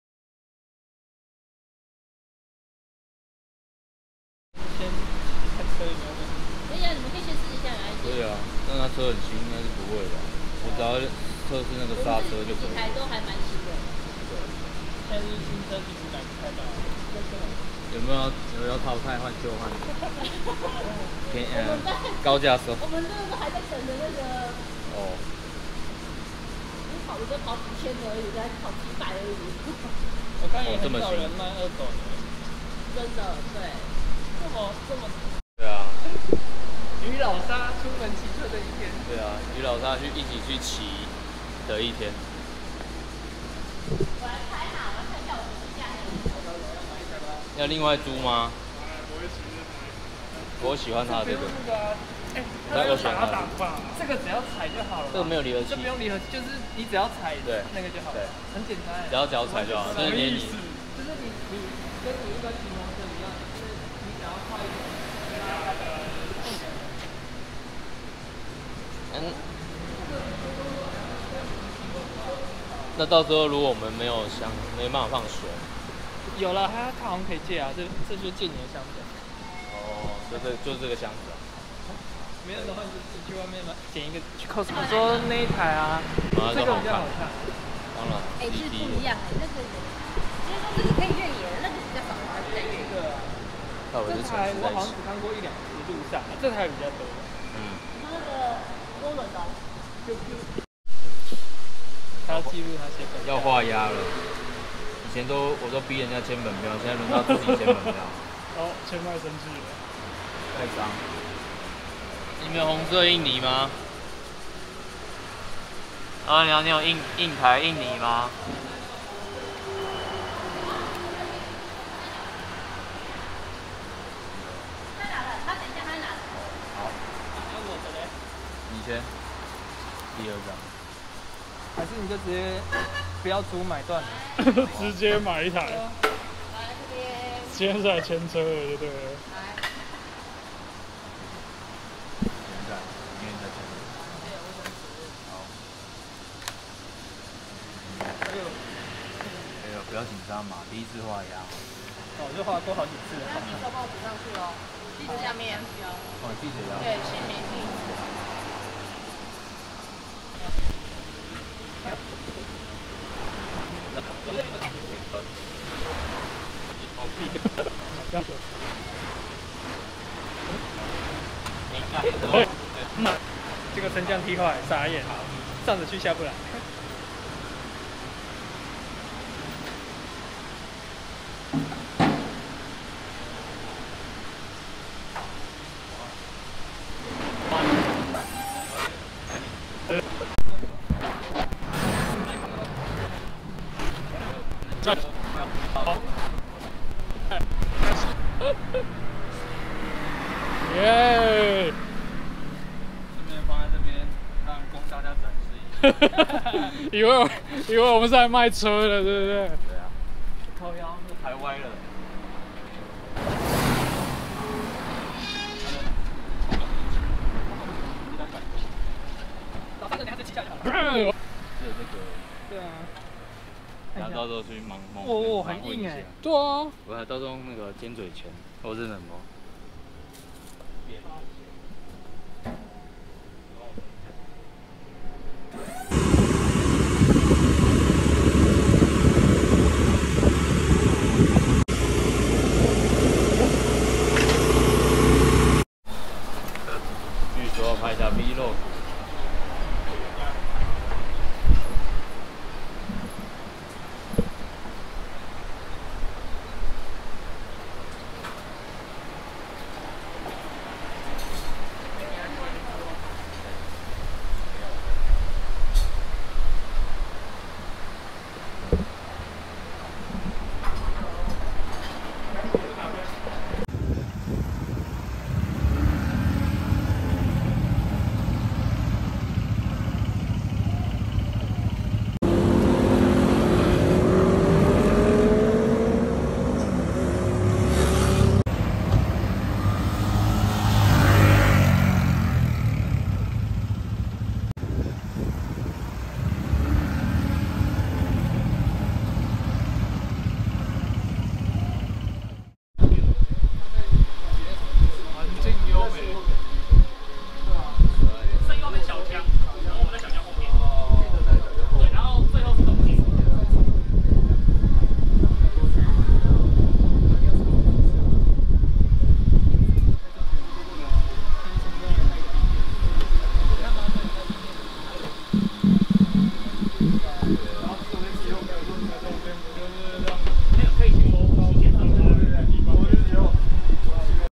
北宜 106 半日遊 | Honda CT125 租車新體驗
不過就是隨手line一下同學有沒有要賣ktr,
結果變成約一約去租ct125來騎。

特地下中壢去車行載回來臺北騎,
光綁車上車就先了1小去了。

石碇走北47·臺9線到坪林,
再回石碇接106上十分.
2趟路差不多剛好100km,
昨天又正好大暑節氣,
熱死我的毛。